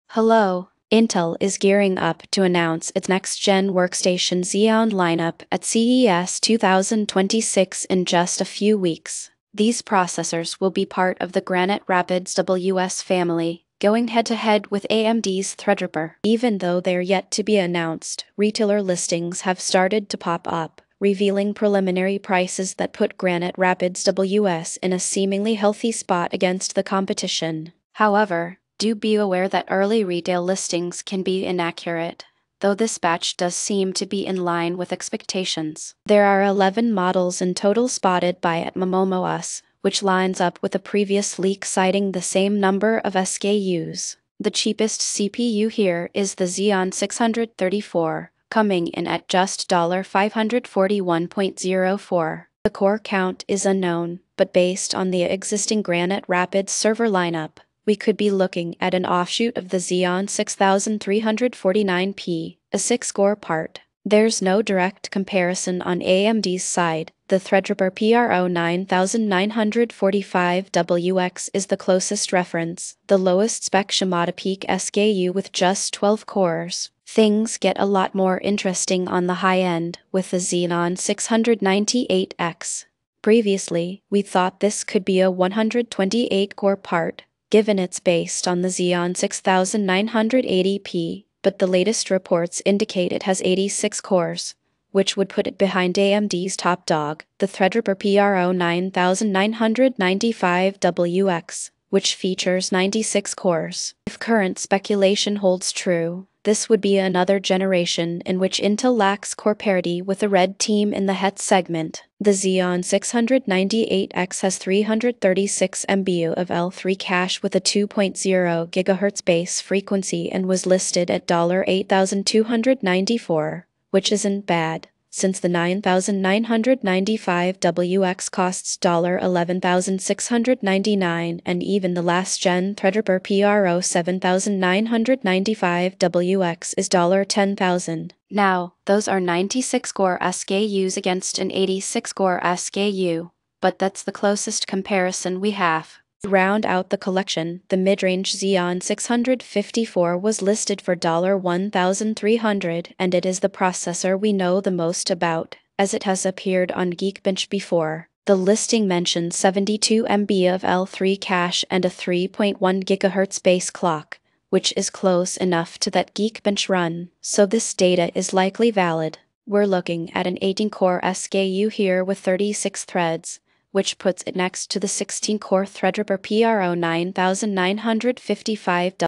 Clip Explains Intel's upcoming Xeon Granite Rapids workstation lineup leaks...
NOT AI-GENERATED.

Intel is gearing up to announce its next-gen workstation Xeon lineup at CES 2026 in just a few weeks. These processors will be part of the Granite Rapids-WS family, going head-to-head with AMD's Threadripper.